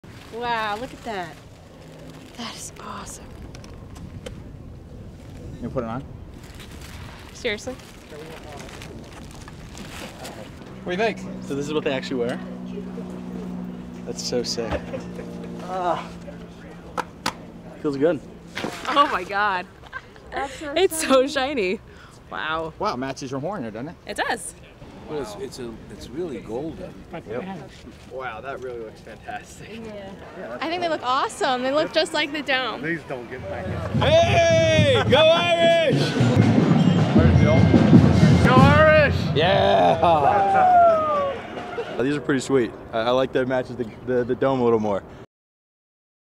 What do you think of the new helmets?
Fans get a first glimpse of the new Notre Dame helmets the day before the ND/USC game.